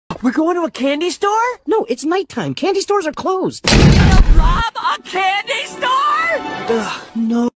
We’re going to a candy store?!fypシ (New members!i will show the other two tomorrow!)
Tbh I’m the only person in this channel who has a giant habit about candy but I wanted to show one of our new character kanaka😭